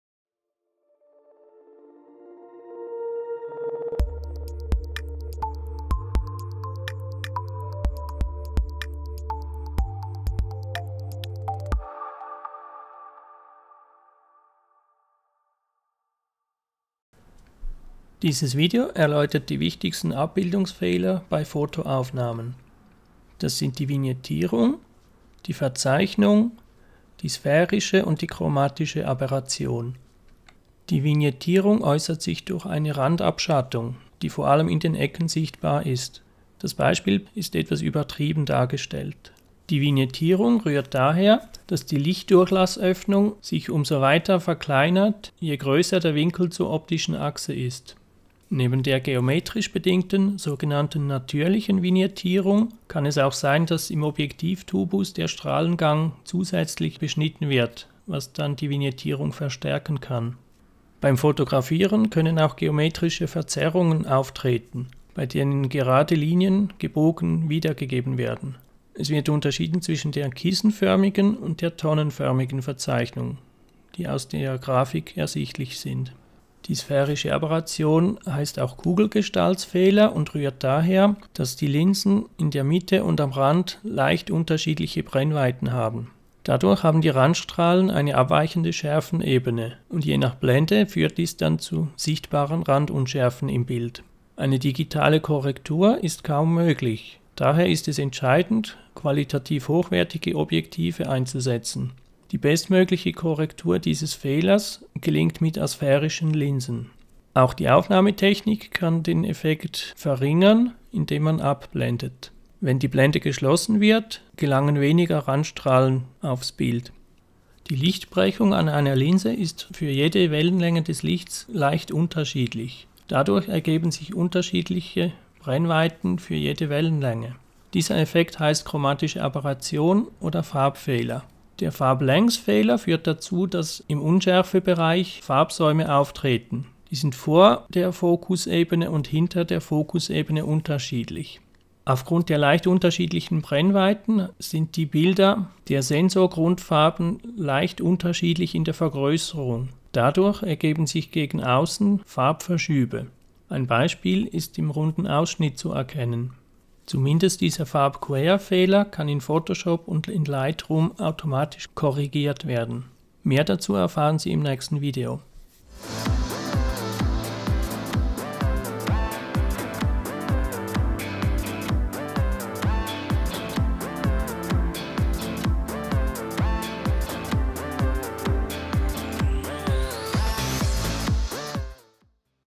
Abbildungsfehler von Objektiven
Objektive sind selten perfekt; hier lernen Sie die häufigsten Abbildungsfehler (er)kennen.